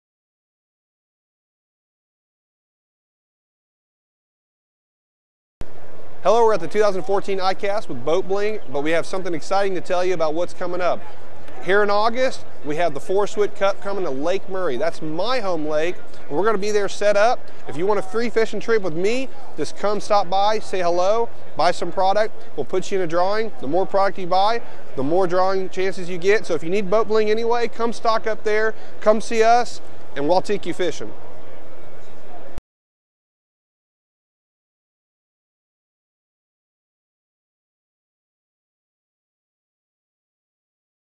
Michael Murphy Contest Announcement
Pro angler Michael Murphy announces Boat Bling's raffle contest to be held during the Forrest Wood Cup in Columbia, SC.